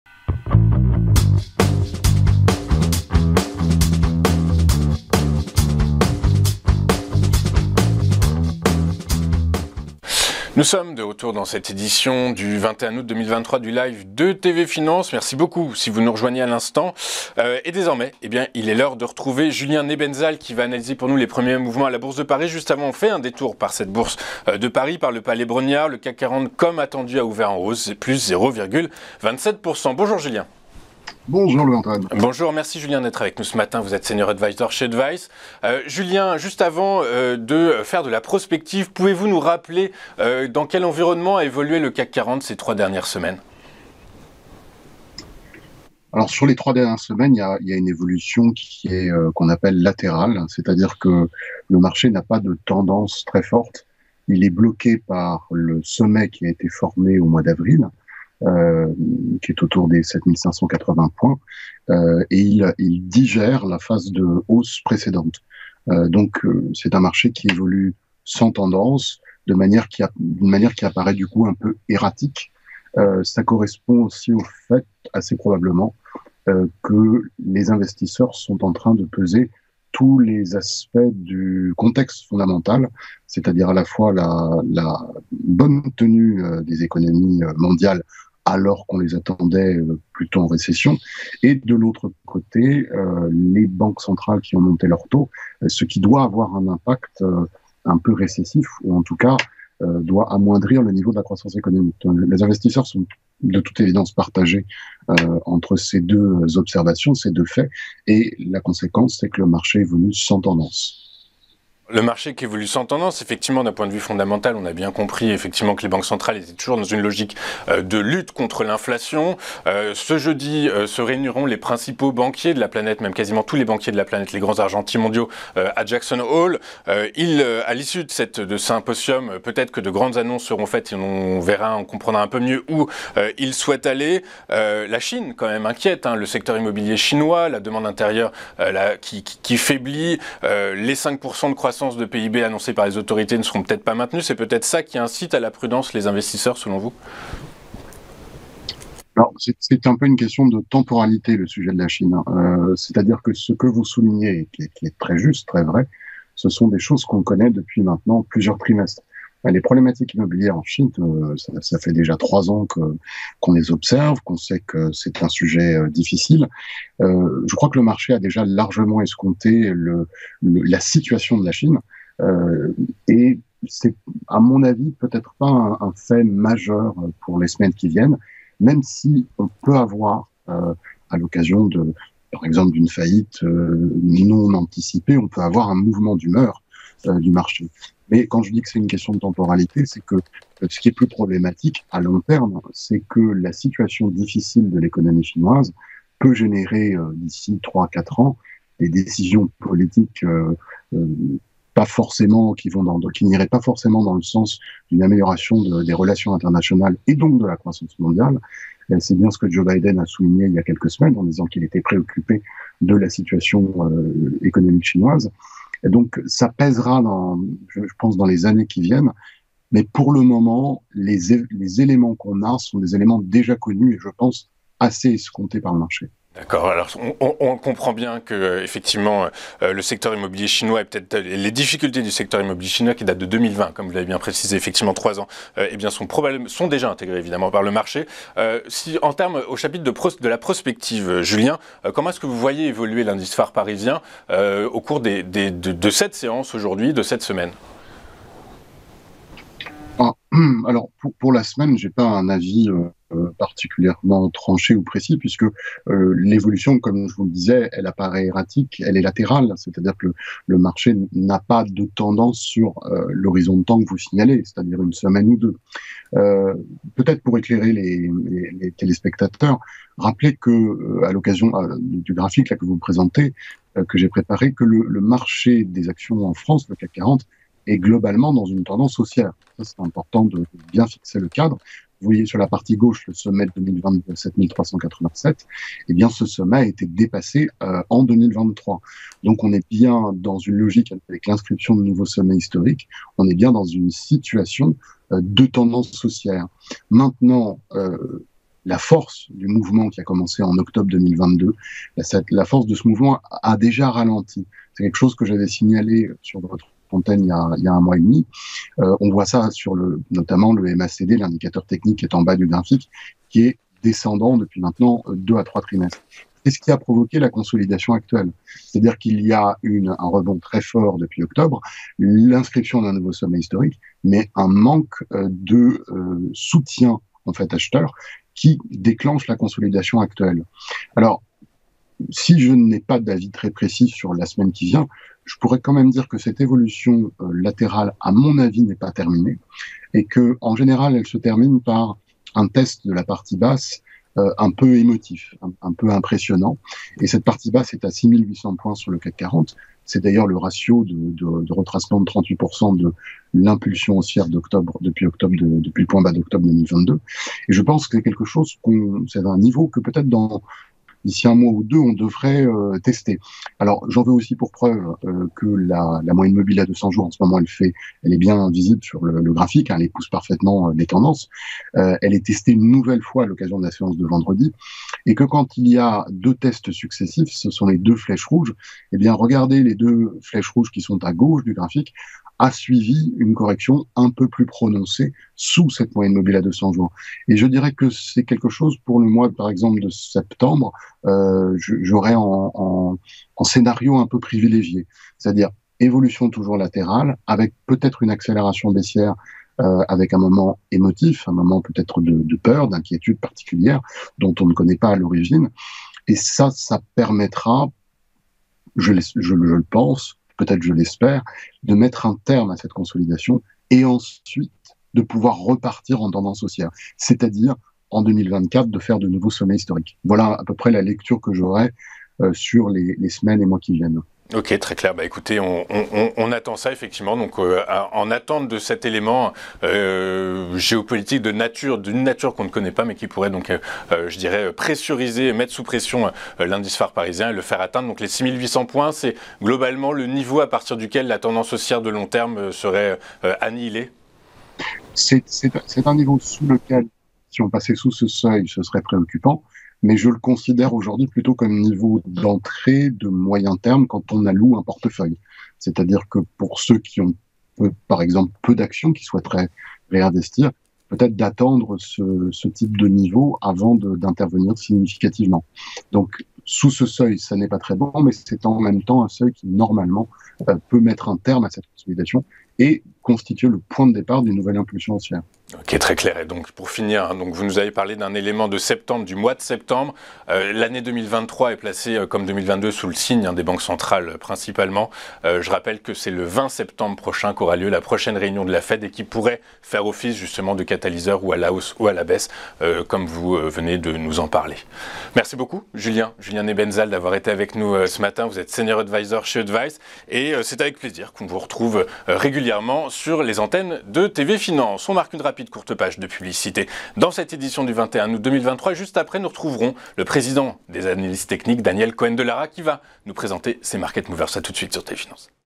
Julien Nebenzahl : « CAC 40 : Marché sans tendance »
Le marché est actuellement sans tendance. Le mouvement de fond reste haussier mais il convient de surveiller avec attention le support majeur calculé à 6 800 points qui pourrait faire office de point pivot et annoncer un changement de paradigme. Pour les prochaines séances le mouvement de consolidation devrait se poursuivre.

Selon notre expert Julien Nebenzahl, Responsable de l'allocation, Advize « l'expert de l'épargne digitale »